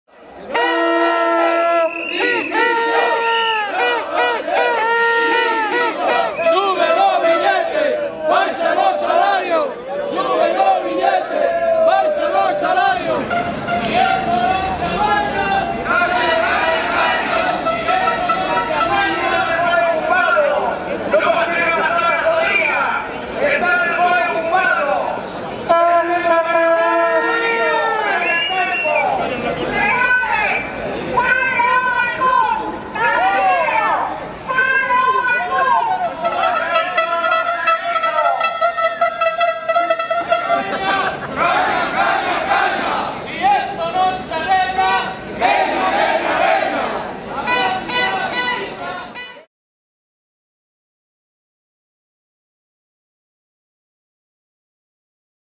2ª xornada de folga en Vitrasa-Vigo (26/11/12)
Os traballadores/as da concesionaria de autobuses urbanos de Vigo realizaron unha protesta diante do Concello de Vigo na segunda xornada de folga en Vitrasa.